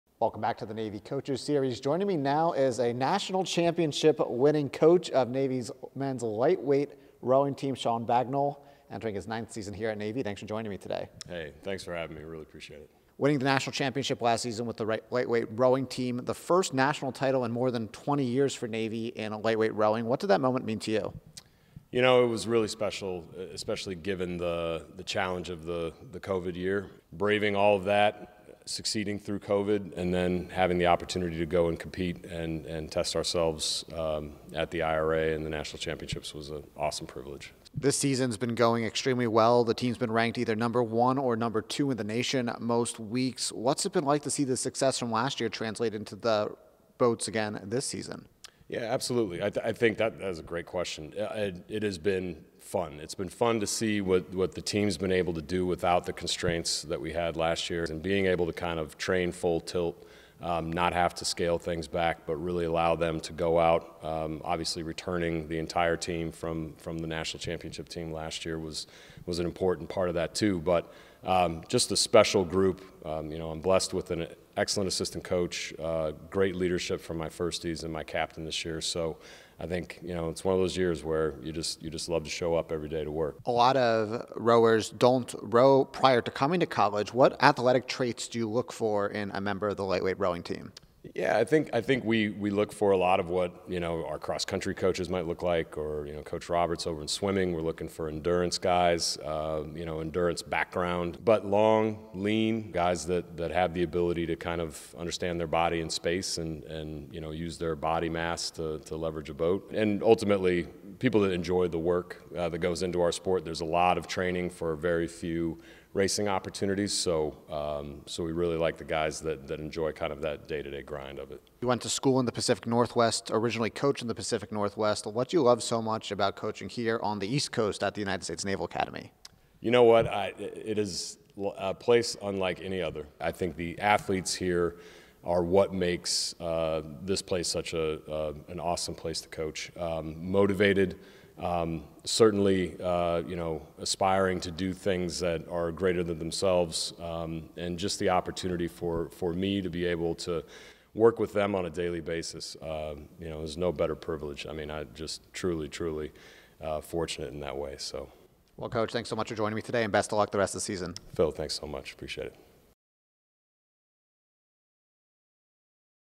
The Navy Coaches Series: Shawn Bagnall
Men's Lightweight Rowing Head Coach Shawn Bagnall sat down with Phil Bergman on this segment of the Navy Coaches Series.

Presented by Towne Transport